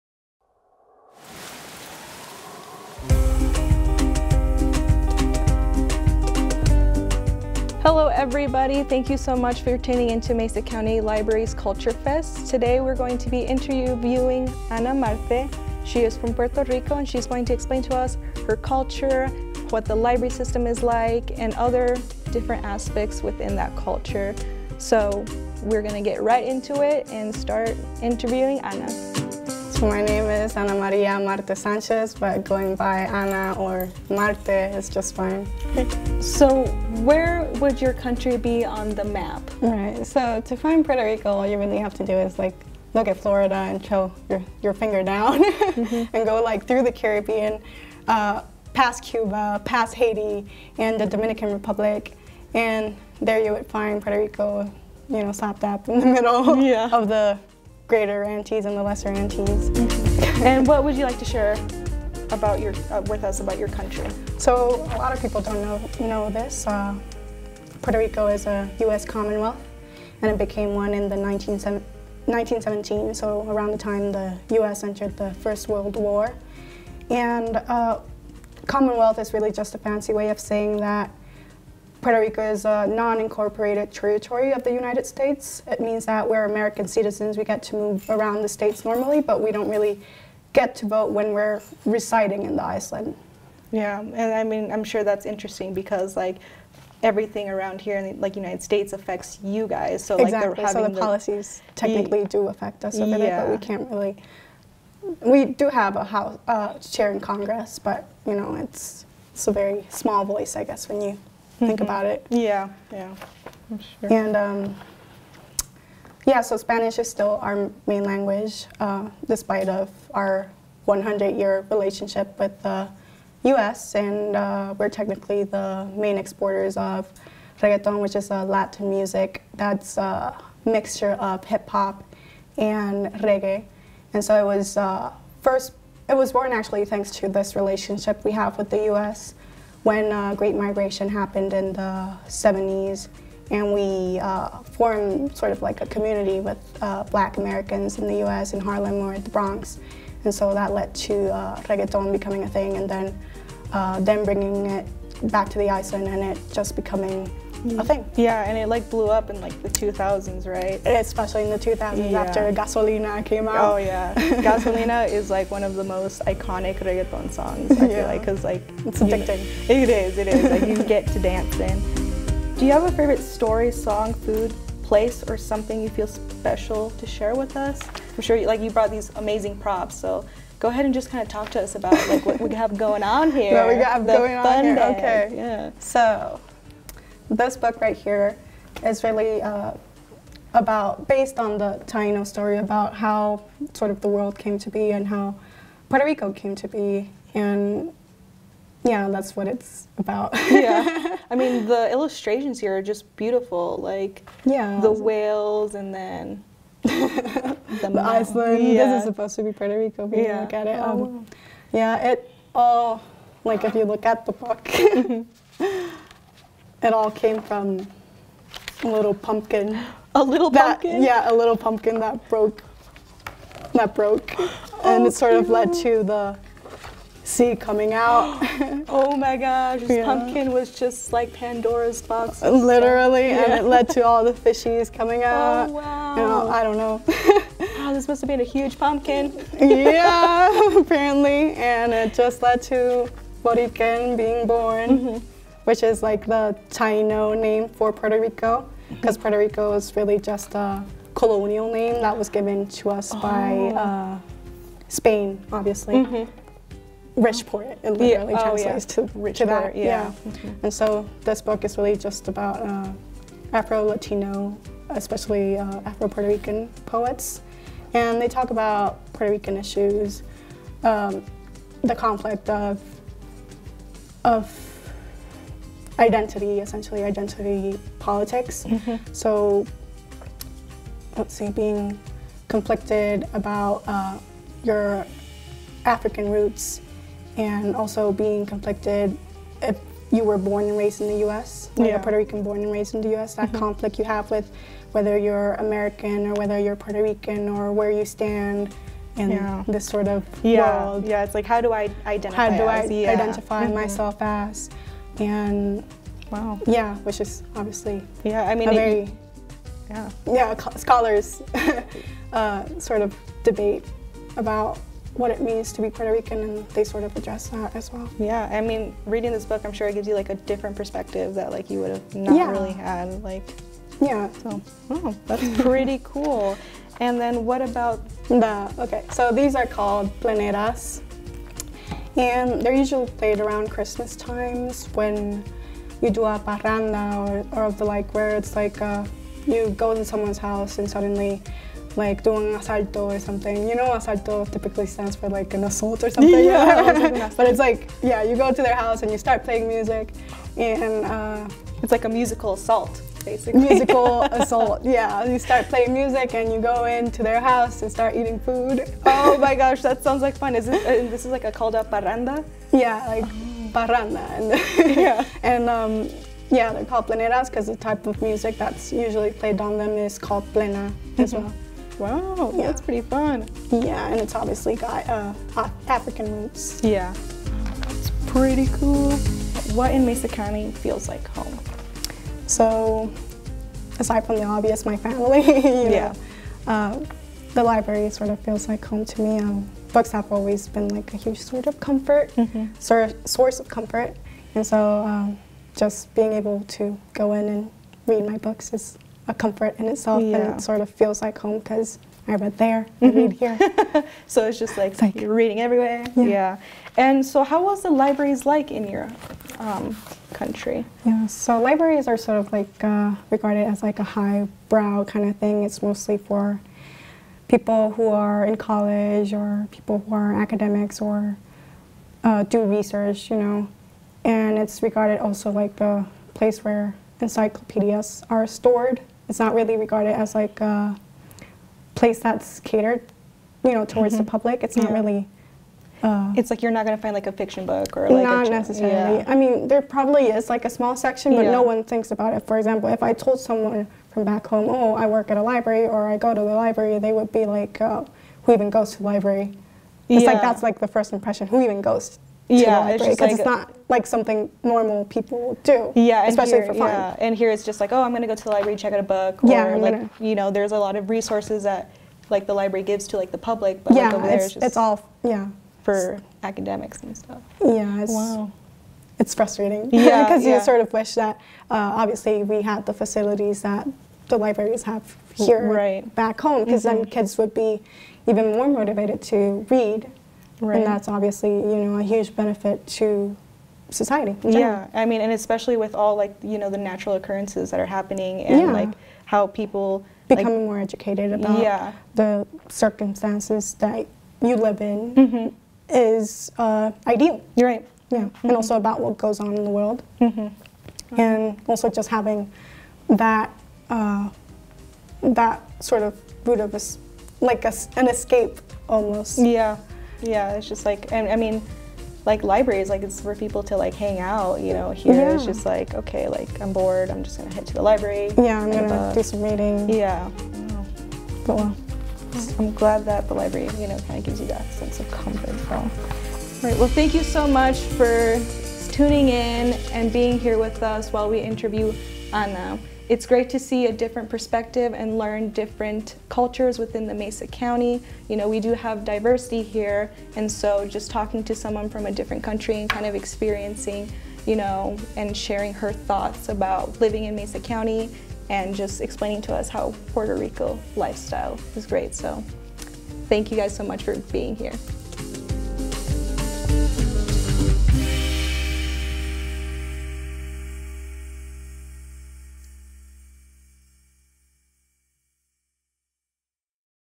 Interview with Ana Marte (Puerto Rico) - Mesa County Libraries Culture Fest
Ana is a native of Puerto Rico who came to Grand Junction after graduating from the University of Puerto Rico, Mayagũez, in 2018 and now works for Mesa County Libraries.

Culture Fest, Mesa County Libraries’ annual celebration of cultural diversity on the Western Slope, happens online this year and features a wide-ranging look at the stories of several immigrants to the region.

for more information about Culture Fest.